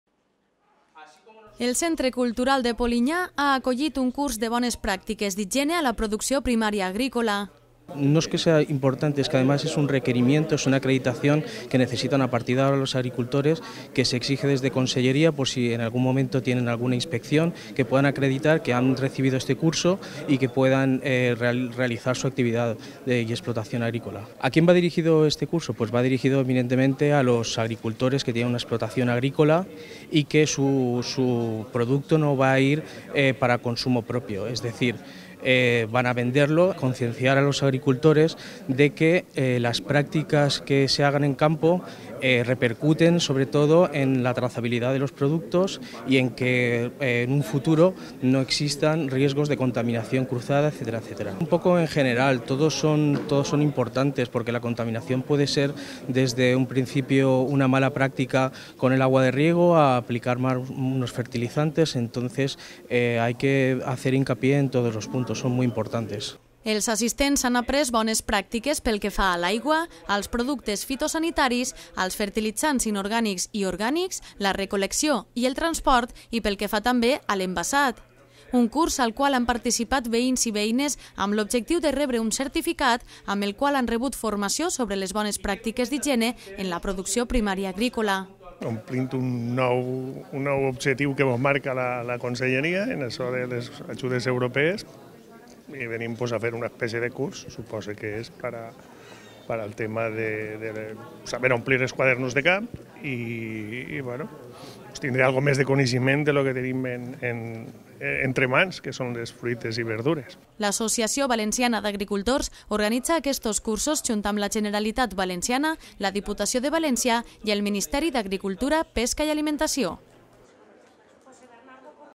Curs de bones pràctiques d’higiene a la producció primària agrícola a Polinyà
S’ha celebrat al Centre Cultural.